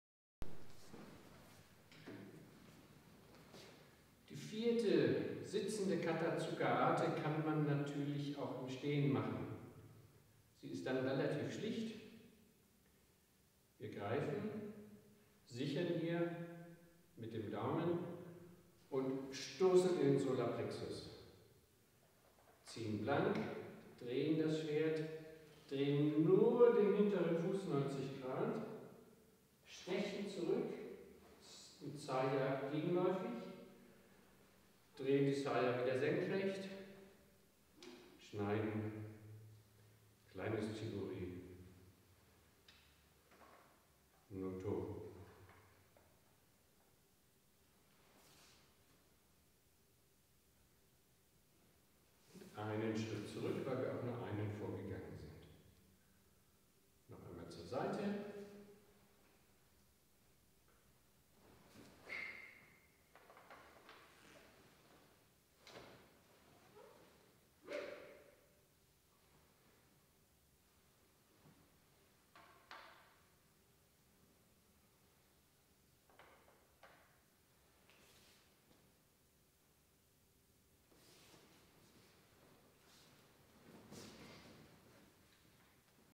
Seitei Iaido Kata 4 Tsuka Ate im Stand
Für diejenigen, die nicht knien können.